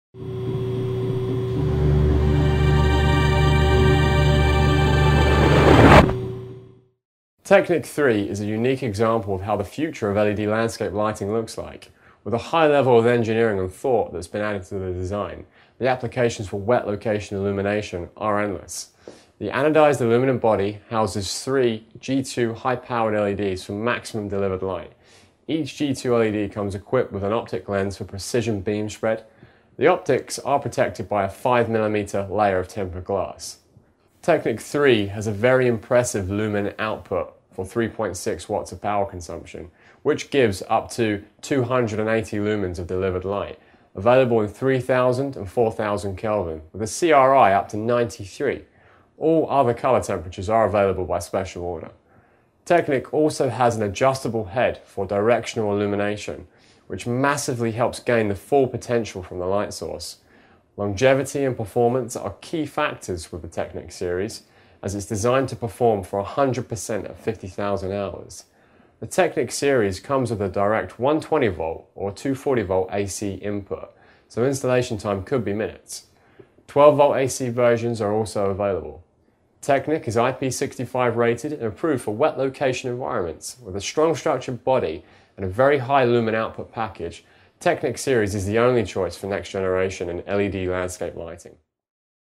MODA LIGHT Product Video - TEKNIK 3
TEKNIK 3 High Powered LED Landscape Fixture. 3 x 1.2 watt LEDs. Contemporary Urban Design for a Professional Landscape Finish. High Power Illumination. Replaces Halogen and Fluorescent. Drain Holes Have Been Added to the New Design to Eliminate any Water Build Up. Mounts Directly to a North American Junction Box for Rapid Installation. Suitable for Exterior Flood Light, Sign Light, Shrub and Small Tree Illumination